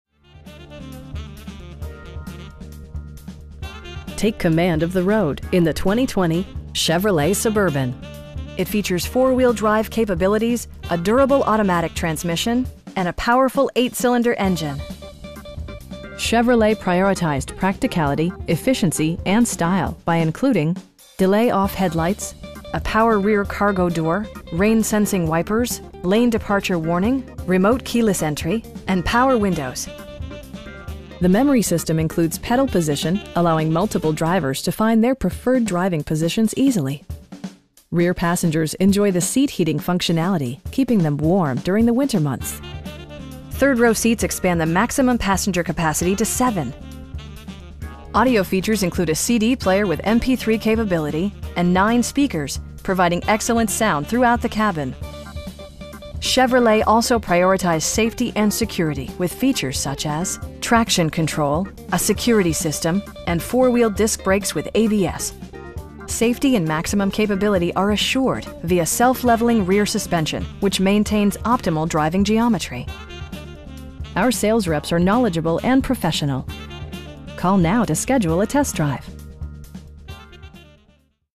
2020 Chevrolet Suburban LT in San Diego, CA 92111
Take command of the road in the 2020 Chevrolet Suburban It features four-wheel drive capabilities, a durable automatic transmission, and a powerful 8 cylinder engine. Chevrolet prioritized practicality, efficiency, and style by including: delay-off headlights, automatic dimming door mirrors, a power rear cargo door, rain sensing wipers, lane departure warning, remote keyless entry, and power windows. Features such as automatic climate control and leather upholstery prove that economical transportation does not need to be sparsely equipped. The memory system includes pedal position, allowing multiple drivers to find their preferred driving positions easily. Rear passengers enjoy the seat heating functionality, keeping them warm during the winter months. Third row seats expand the maximum passenger capacity to 7. Audio features include a CD player with MP3 capability, steering wheel mounted audio controls, and 9 speakers, providing excellent sound throughout the cabin. Chevrolet also prioritized safety and security with features such as: head curtain airbags, front side impact airbags, traction control, brake assist, a security system, and 4 wheel disc brakes with ABS. Safety and maximum capability are assured via self leveling rear suspension, which maintains optimal driving geometry. Our sales reps are knowledgeable and professional. We'd be happy to answer any questions that you may have.